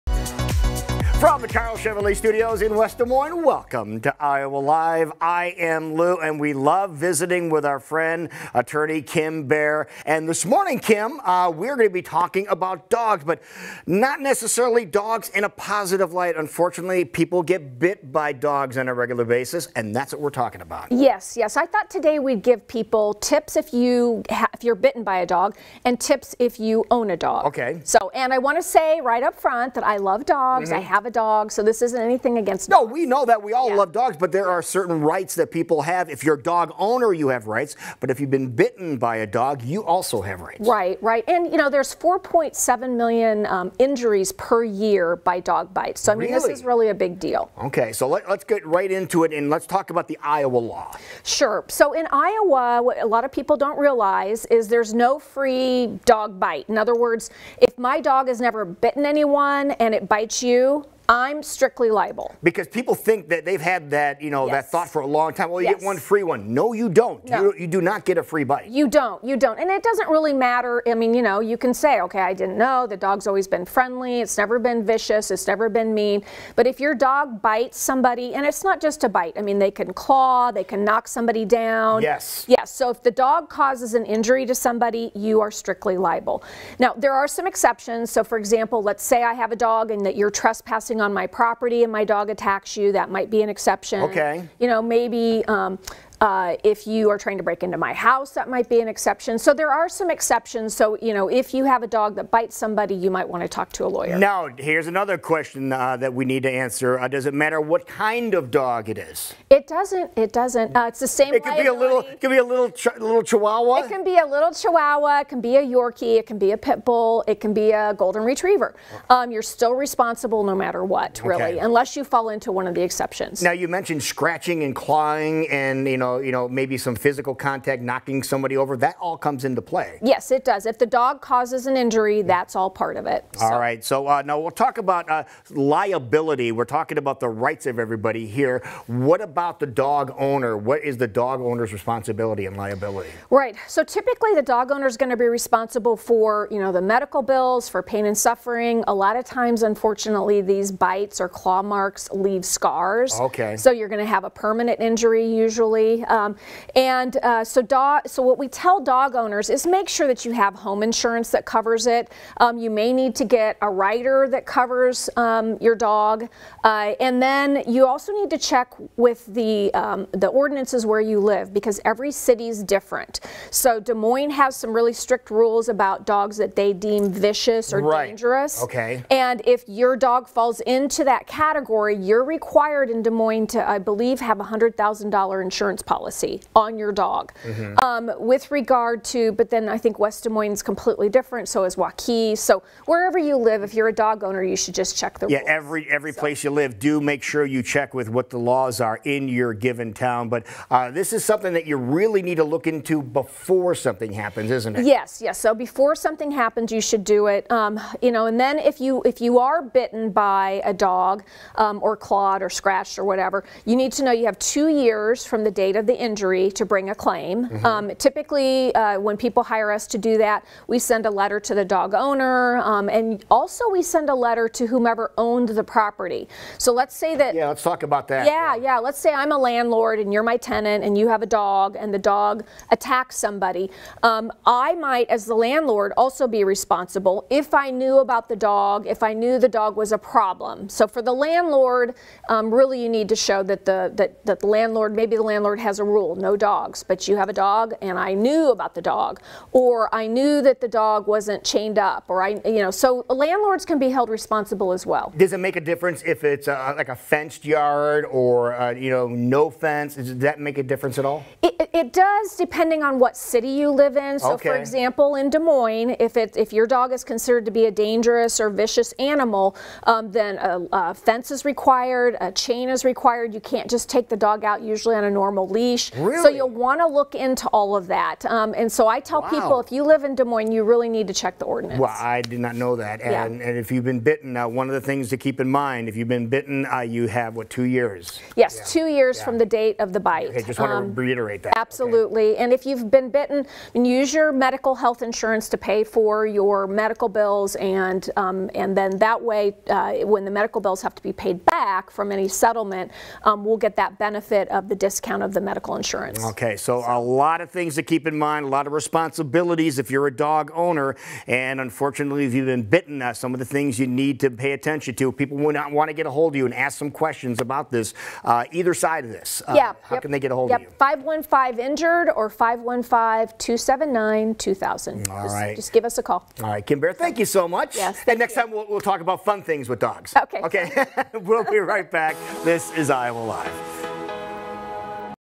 What you need to know when a dog bites | Paid Content
Attorney Kim Baer discusses what you need to know if you are bitten, own a dog who has bitten or attacked someone or a property owner with tenants | Paid Content